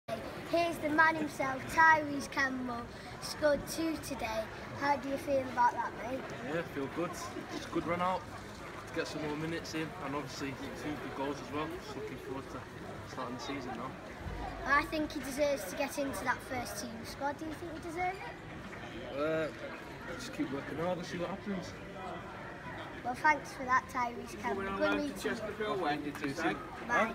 "I think he deserves to get into that first team" | SAMCAM CHATS TO TYRESE CAMPBELL
It's almost a year to the day since I originally posed this question to @TyreseKCampbell. After scoring another brace in our pre-season friendly victory over Lincoln City tonight, making it a total of 5 in 3 games following on from a highly successful spell on loan at Shrewsbury Town, I believe his time may have finally arrived.

What do you think... Will Tyrese follow in Tom Edwards' footsteps into the first team?

"TYRESE CAMPBELL... HE'S ONE OF OUR OWN!!"